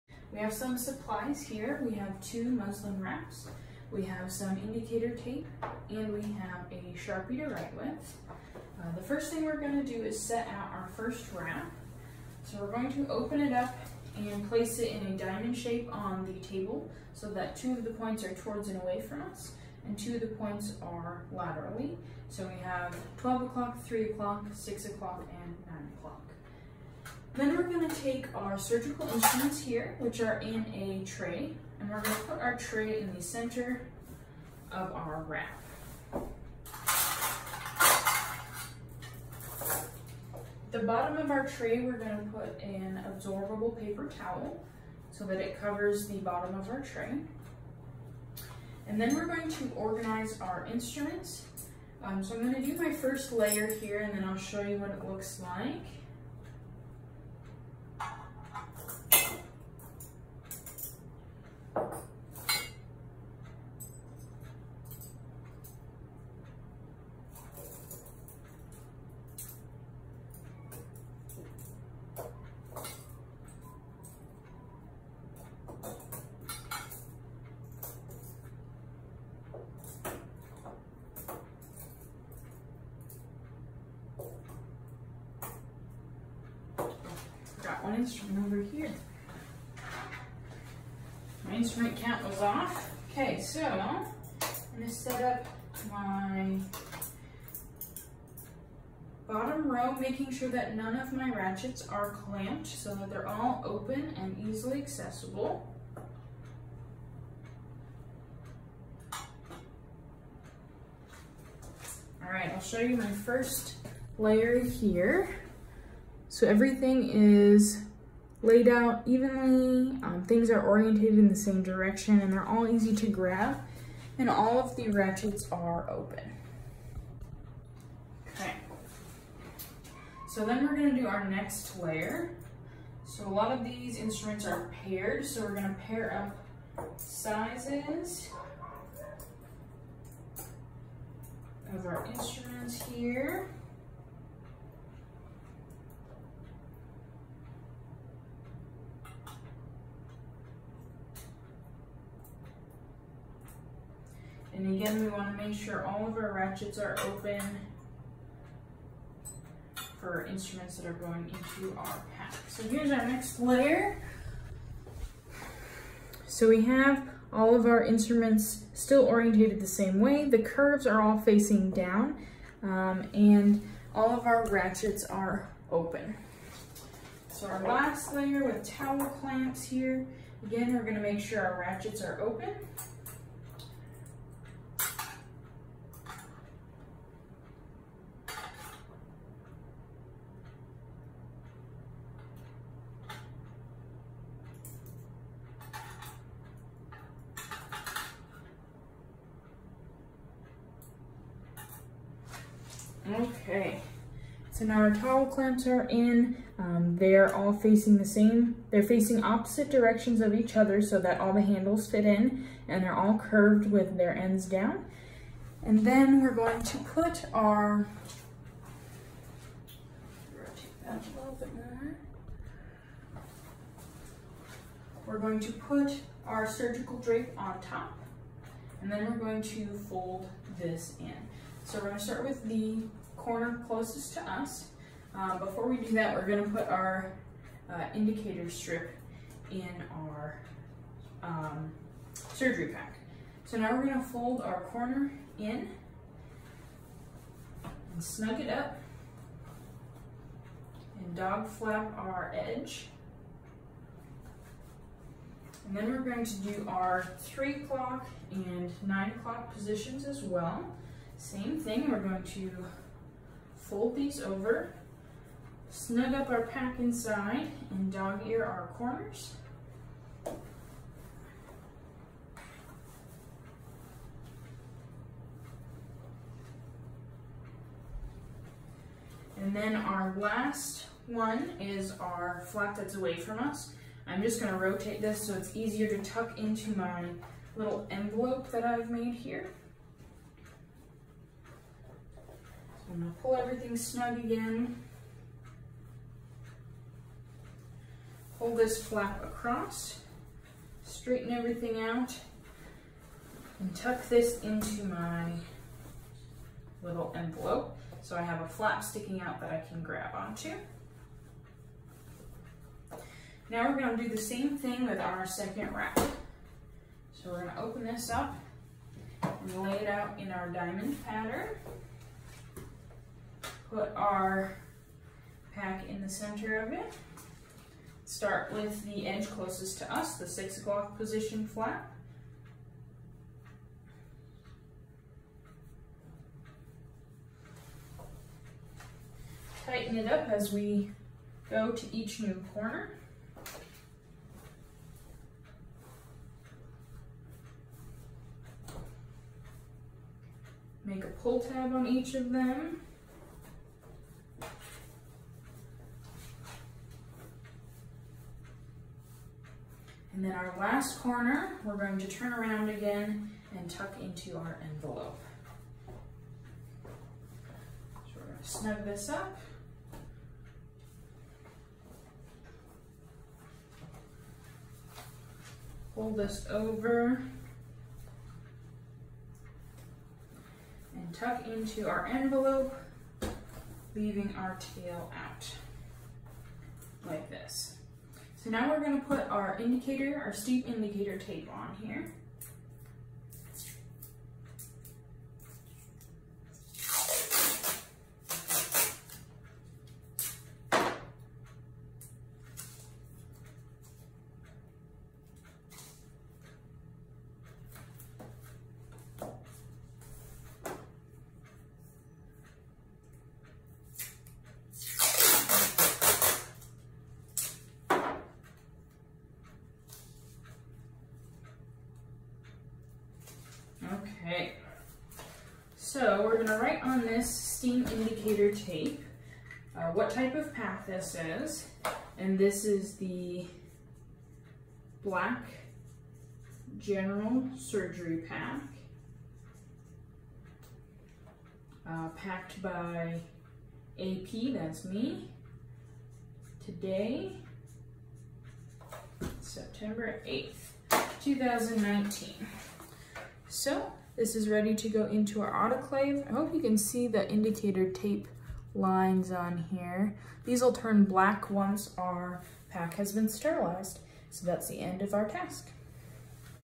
Wrapping a surgical pack
SOP Description
Wrapping Surgical Packs
Job Level (Who should be able to perform the task)
Veterinary Technicians
Veterinary Assistants
Kennel Staff
Special Skills Needed
Attention to detail
Knowledge of different types of surgery packs
Approximate Time Needed
15-20 minutes per pack
Equipment and Supplies   MSDS Required YES  NO
Surgical wraps                                                                          *
Surgical towels                                                                                                                         *
4x4 gauze                                                                                                                                  *
Indicator tape                                                                                                              *
Paper towel                                                                                                                              *
Masking tape                                                                                                                          *
Indicator strip                                                                                                                           *
Black sharpie            *
Metal surgical tray           *
Surgical instruments           *

Instructions (step-by-step)

1. Gather supplies - two washed and lint rollled wraps, four washed, lint rolled, and accordion folded surgical towels, 10 pieces of 4x4 gauze, indicator tape, masking tape, indicator strip, black sharpie, surgical tray, instruments, and paper towel
2. Lay first wrap on clean, dry surface so that there is a point directly facing you and the wrap is in a diamond shape.
3. Set your surgical tray in the center and organize the instruments in the pack:
a. Lay a paper towel in the bottom of the surgical instrument tray
b. Inspect that all instruments are clean and ready to be packed.
c. Lay out your first layer of instruments as depicted in the instrument photo book. These will be your scissors, needle drivers, thumb forced type instruments. This will get updated from time to time as our surgical packs change
d. Lay out your second layer of instruments as depicted in the instrument photo book. These will be your tissue forceps and hemostats. Make sure all ratchets are in the open position and that all curved tips are facing down.
e. *not shown in video* apply two stacks of 4x4 gauze (5 gauze in each stack) for the third layer
f. Lay your towel clamps on top of the 4x4 gauze, again making sure all the ratchets are open and the curved tips are facing down. This will be layer four.
g. Layer five will be the surgical drape. Instead of using a disposable blue drape as depicted in the video, please use 4 of the green surgical towels. These should be placed on top of the towel clamps in two stacks of two.
h. Apply an indicator strip on top of the towels
• 4. Start wrapping your surgical tray by folding the 6:00 corner of the wrap over the tray and dog ear fold the point. Then fold the 3:00 and 9:00 corners in the same method, making sure the pack in snug and there are no wriggles in the wrap.
• 5. The last corner (the 12:00) corner with be tucking into the envelope made by the first three corners and the tip of the corner should be left to grab onto when opening the pack.
• 6. Repeat the wrapping steps to double wrap the surgical pack in a second wrap.
• 7. After the pack is double wrapped, apply a strip of masking tape across the opening of the pack and add a courtesy tab to each end of the tape. Then add a 2 inch strip of indicator tape to allow for a quick sterilization check.
• 8. Write your initials and the date of sterilization (not the date of wrapping) on the masking tape, as well as the pack type and color. Use black sharpies only, other colors will bleed through the masking tape onto the wrap.

Person responsible for additional questions or information
Angie or Michelle